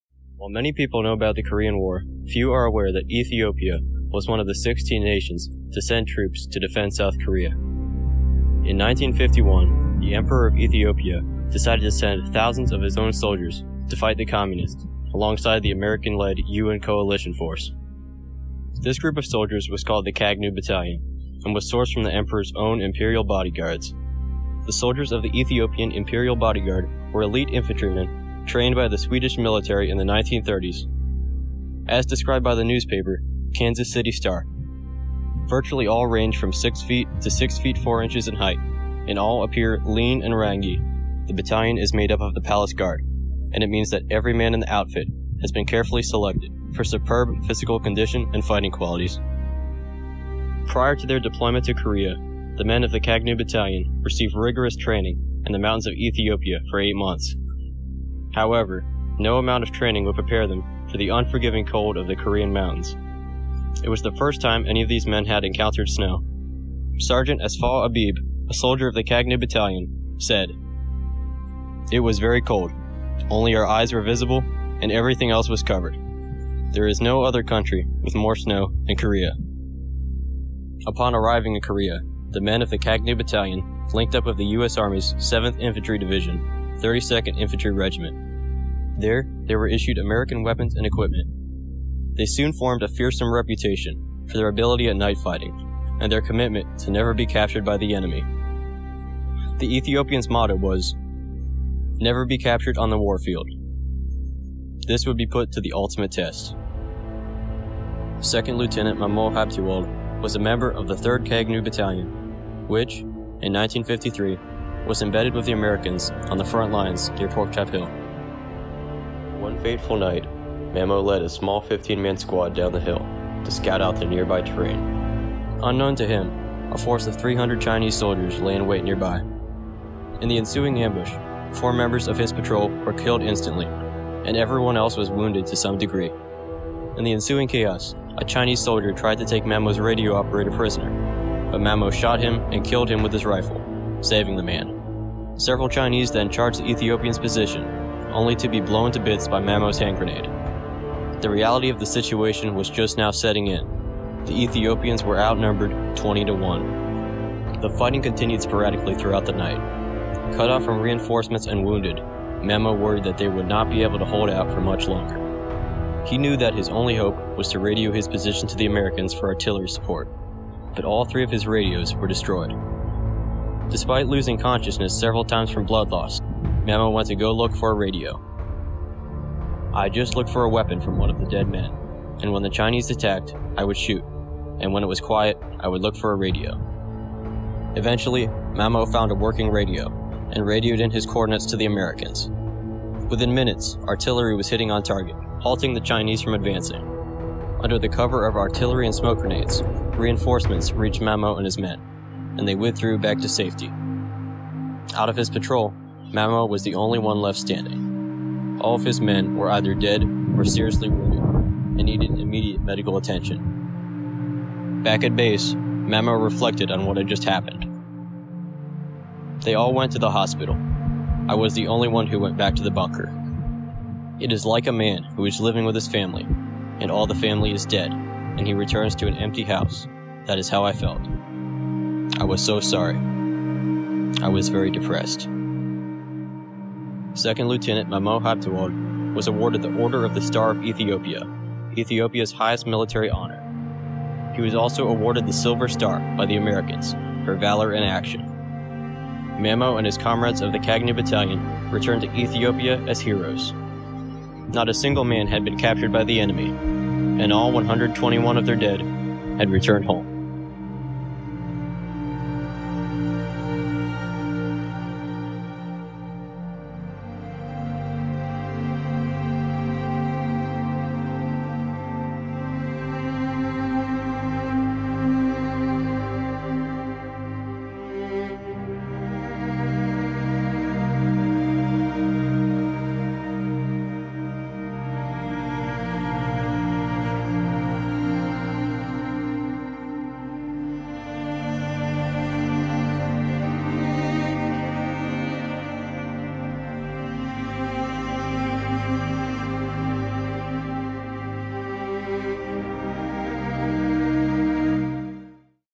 Twenty to One: 2nd LT Mamo Habtewald & the Kagnew Battalion in Korea (ቃኘው) | May 1953
While many people know about the Korean War, not many are aware that Ethiopia contributed to the fight. In 1951, at the start of the Korean War, the Emperor of Ethiopia sent thousands of his own soldiers to fight alongside the American-led UN coalition force against the communist South Korea. These elite infantry units came to be known as the Kagnew Battalion, and soon formed a fearsome reputation for their ability at night fighting and their commitment to leave no man behind. Their motto was "Never be captured on the war field," and none of them were ever captured during the war. One man, 2nd LT. Mamo Habtewold of the Third Kagnew Battalion, displayed extraordinary courage and heroism during the war, when, one fateful night, his patrol was ambushed by a force of Chinese soldiers that outnumbered him twenty to one. This is the story of the events that played out that night and the extraordinary courage and heroism displayed by 2nd LT Mamo Habtewold.

This video is dedicated to 2nd LT Mamo Habtewald and all the men who served in the Kagnew Battalion.

If you enjoyed the video, please like it and share on whatever social media platform you use.